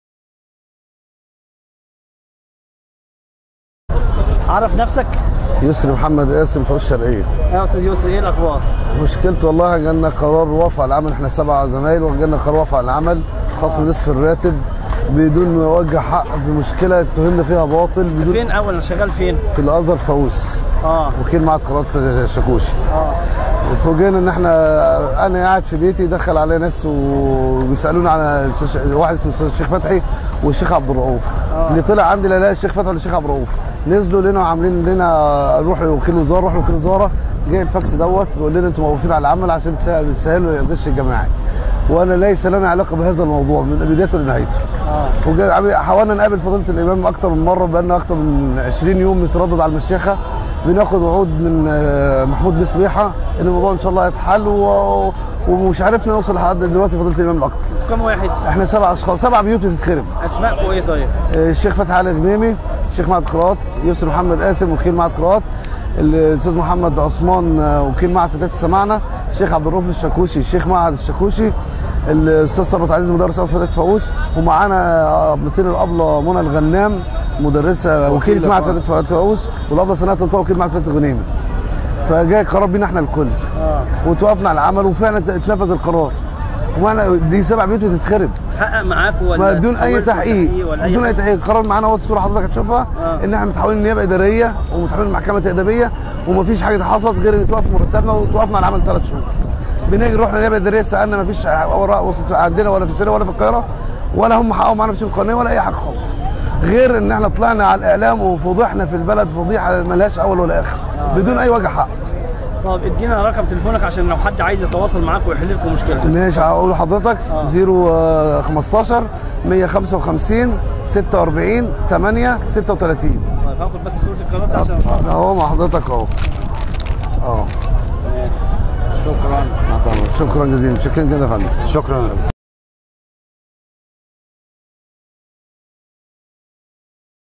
إستغاثة لشيخ الأزهر من بعض العاملين بالأزهر
من ميدان التحرير ظهر يوم الخميس إستغاثة لشيخ الأزهر من بعض العاملين بالأزهر